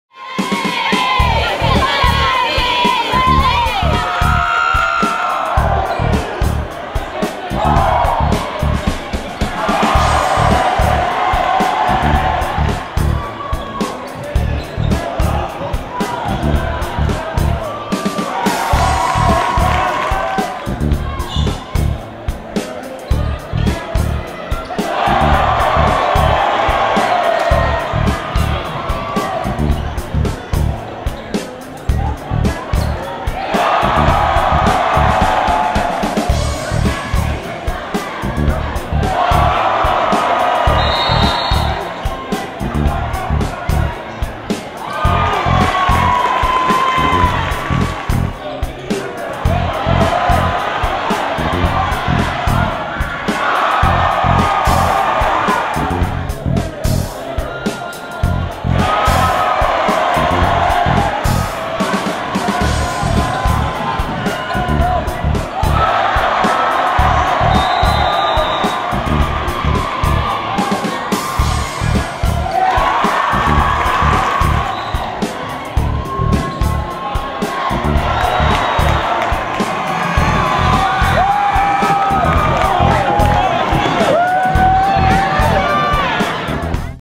Clear Lake-Clear Creek Basketball
The Clear Lake Falcons take down rival the Clear Creek Wildcats in the boy's basketball regular season finale 66-53.